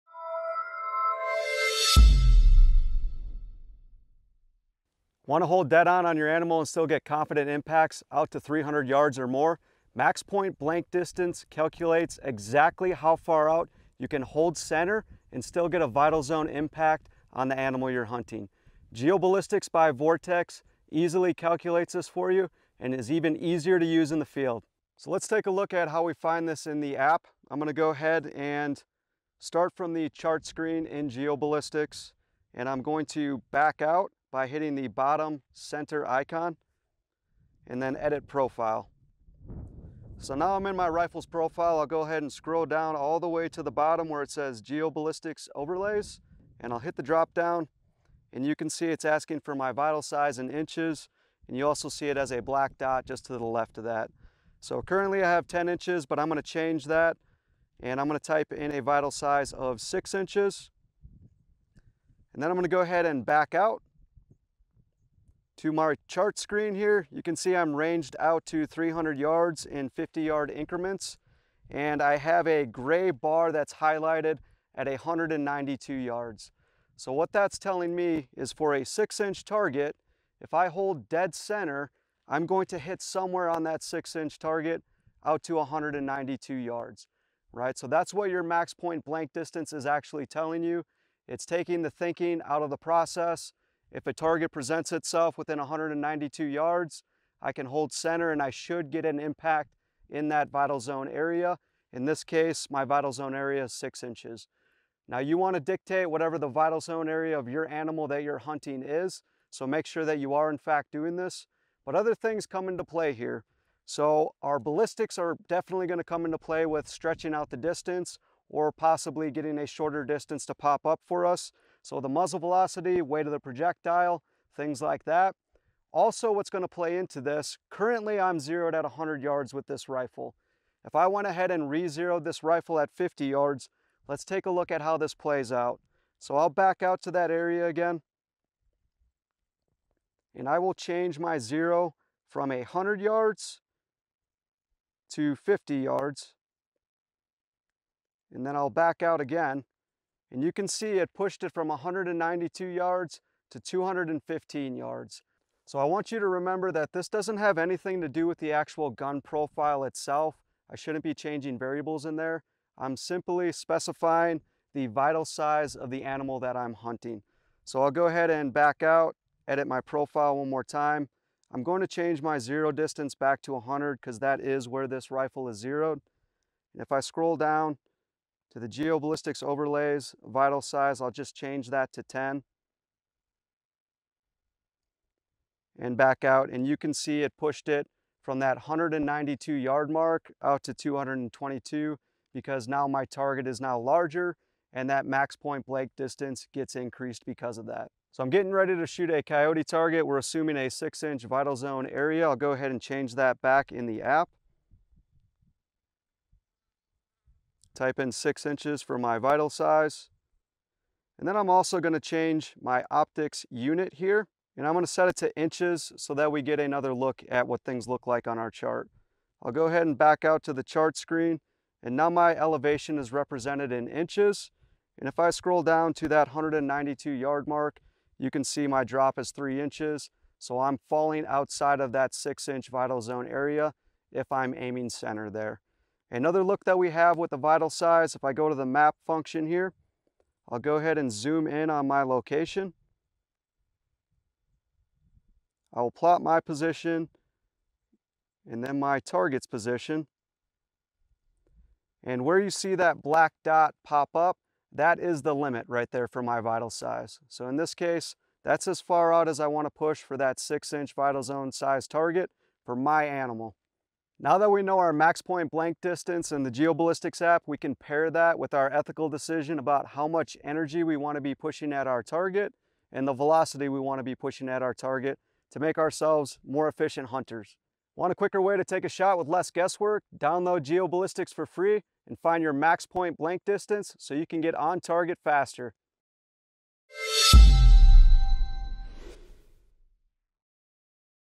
Max Point-Blank Distance Explained | How Far Can You Hold Dead On with GeoBallistics®
Want to hold dead-on and still make consistent hits out to 300+ yards without dialing or holding over? Max Point-Blank Distance (MPBR) tells you exactly how far you can shoot while staying inside the vital zone. In this video, Vortex Edge® Instructor Corey breaks down how GeoBallistics® calculates your MPBR, helps you set the ideal zero, and shows how to use the in-app overlay to simplify fast shots in the field. From ballistic data to bullet drop visualization, learn how to take smarter shots with less guesswork—no turret turns, or mental math required.

Perfect for hunters, backcountry shooters, and anyone who needs speed and confidence on shot opportunities.